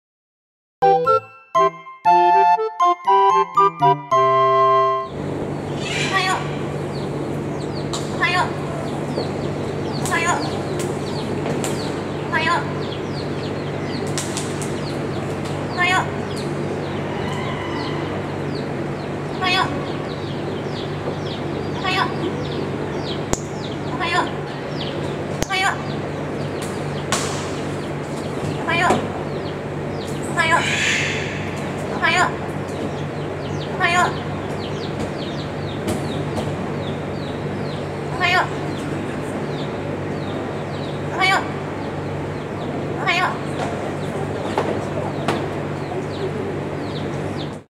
いっぱい挨拶してくれるキビタイボウシインコのぴーちゃん 🐾 草津熱帯圏
草津熱帯圏トロピカルワンダーランドであえます！
ぴーちゃんいっぱい「おはよう」って挨拶してくれた。笑
キビタイボウシインコって賢いんですね(^^)